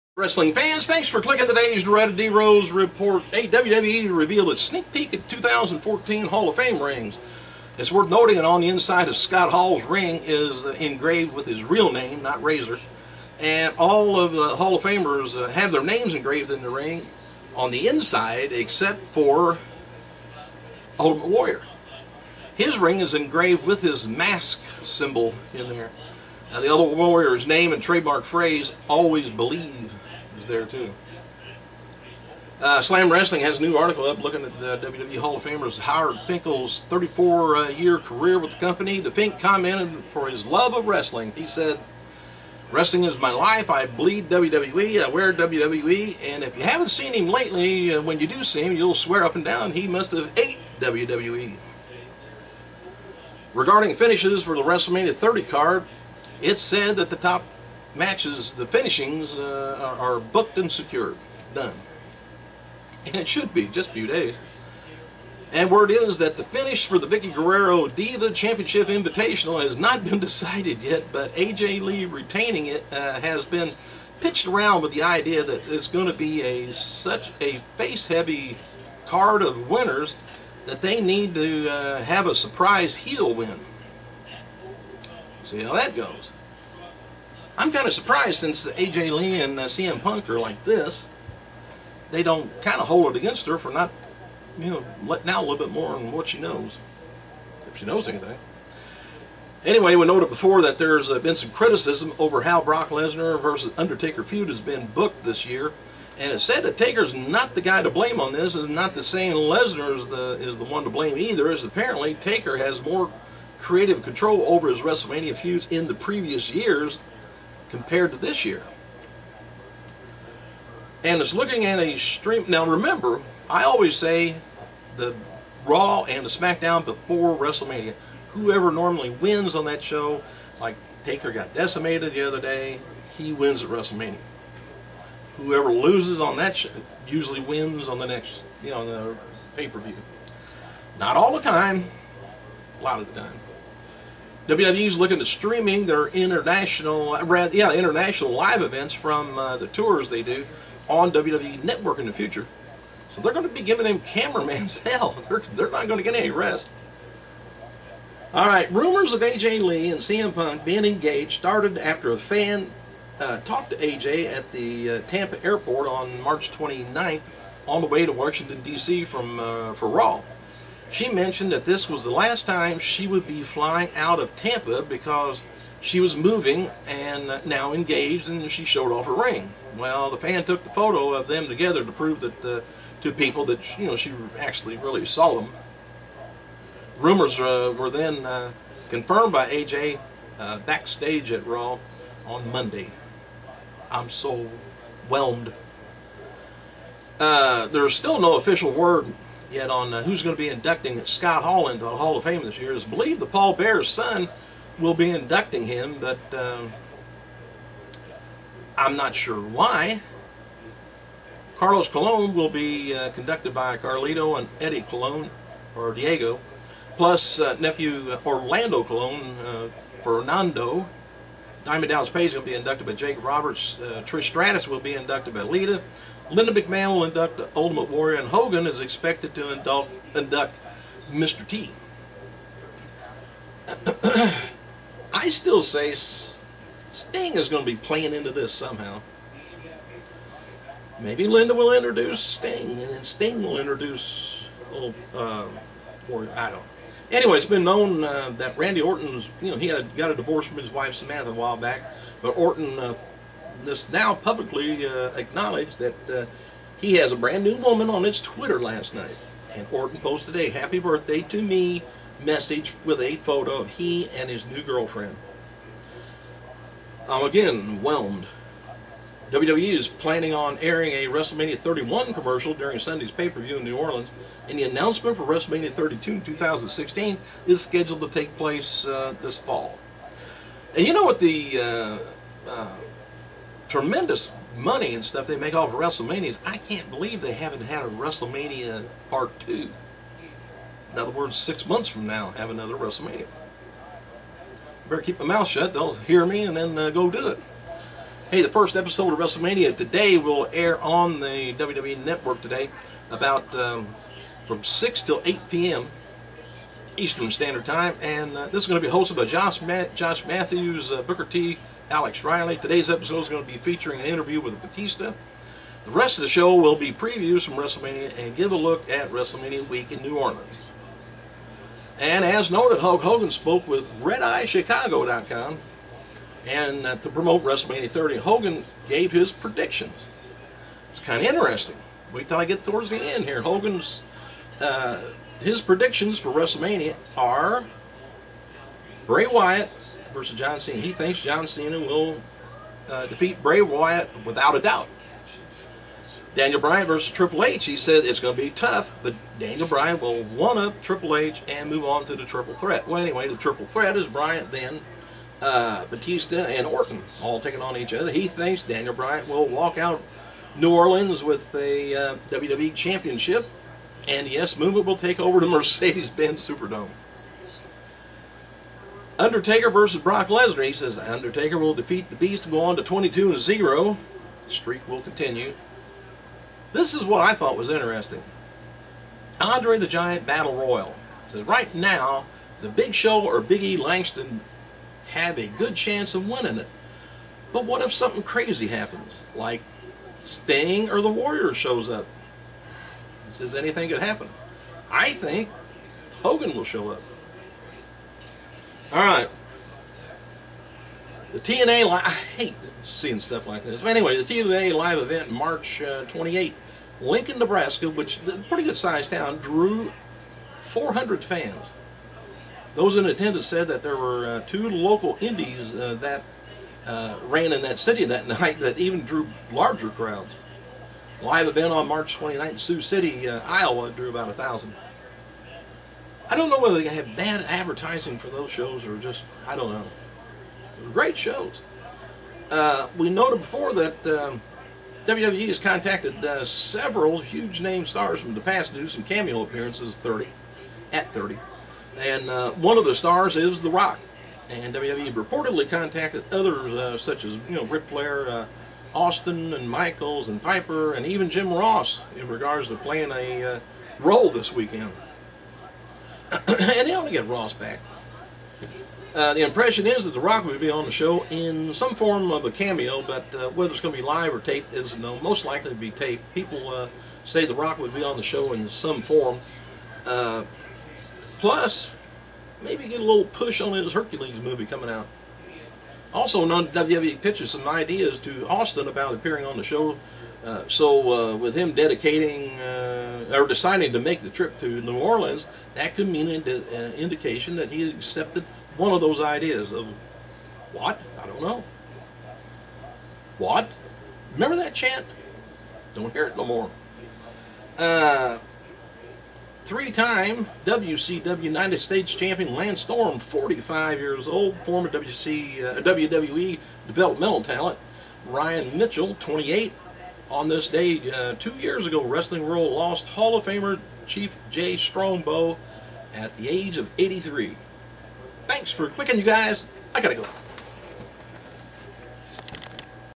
wrestling news dreaded d. rose report tna wwe ovw nxt roh mm
BREAKING NEWS= HOGAN GIVES HIS SURPRISE PREDICTIONS FOR WMXXX. ALL MATCH FOR XXX IS SET EXCEPT 1. DIVA MATCH, WHAT TO DO??? WWE TO STREAM WWE ON TOUR COMING SOON. UPDATE ON HALL OF FAMER RINGS AND WHAT THEY SAY. WILL J.R. BE THERE??? LOTS MORE NEWS, AFTER THE CLICK.